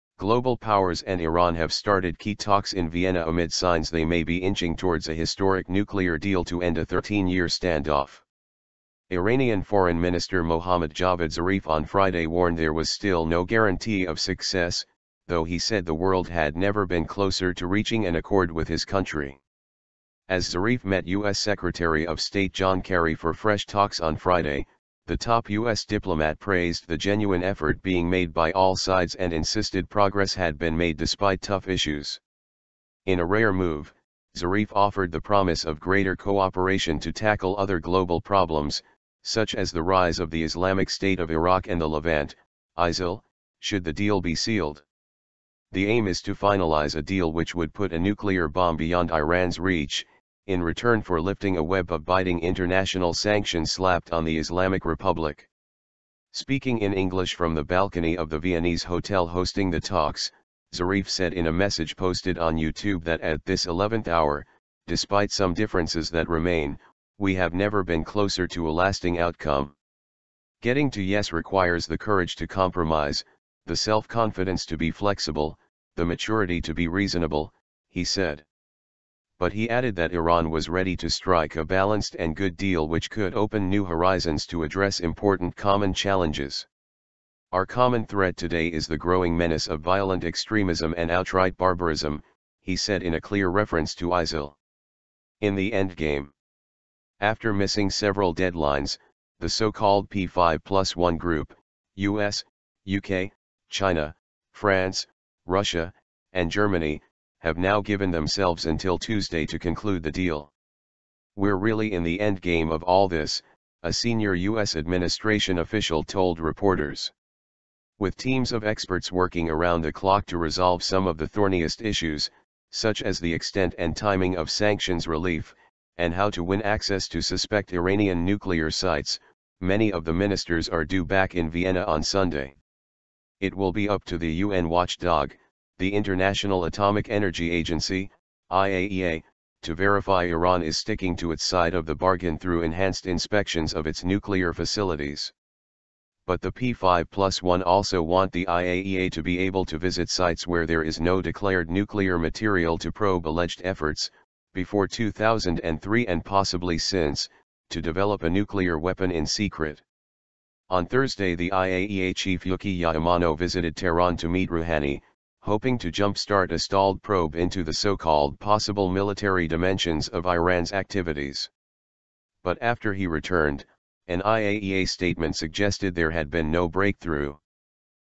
Iran nuclear deal 'never been closer'
Global powers and Iran have started key talks in Vienna amid signs they may be inching towards a historic nuclear deal to end a 13-year standoff.

Iranian Foreign Minister Mohammad Javad Zarif on Friday warned there was still "no guarantee" of success, though he said the world had "never been closer" to reaching an accord with his country.

As Zarif met US Secretary of State John Kerry for fresh talks on Friday, the top US diplomat praised "the genuine effort" being made by all sides and insisted progress had been made despite "tough issues".

In a rare move, Zarif offered the promise of greater cooperation to tackle other global problems, such as the rise of the Islamic State of Iraq and the Levant (ISIL), should the deal be sealed.

The aim is to finalise a deal which would put a nuclear bomb beyond Iran's reach, in return for lifting a web of biting international sanctions slapped on the Islamic republic.

Speaking in English from the balcony of the Viennese hotel hosting the talks, Zarif said in a message posted on YouTube that at "this 11th hour, despite some differences that remain, we have never been closer to a lasting outcome".

"Getting to yes requires the courage to compromise, the self-confidence to be flexible, the maturity to be reasonable," he said.

But he added that Iran was ready to strike "a balanced and good deal" which could "open new horizons to address important common challenges".

"Our common threat today is the growing menace of violent extremism and outright barbarism," he said in a clear reference to ISIL.

'In the endgame'

After missing several deadlines, the so-called P5+1 group - US, UK, China, France, Russia, and Germany - have now given themselves until Tuesday to conclude the deal.

"We're really in the endgame of all this," a senior US administration official told reporters.

With teams of experts working around the clock to resolve some of the thorniest issues - such as the extent and timing of sanctions relief, and how to win access to suspect Iranian nuclear sites - many of the ministers are due back in Vienna on Sunday.

It will be up to the UN watchdog, the International Atomic Energy Agency (IAEA), to verify Iran is sticking to its side of the bargain through enhanced inspections of its nuclear facilities.

But the P5+1 also want the IAEA to be able to visit sites where there is no declared nuclear material to probe alleged efforts, before 2003 and possibly since, to develop a nuclear weapon in secret.

On Thursday the IAEA chief Yukiya Amano visited Tehran to meet Rouhani, hoping to jumpstart a stalled probe into the so-called "possible military dimensions" of Iran's activities.

But after he returned, an IAEA statement suggested there had been no breakthrough.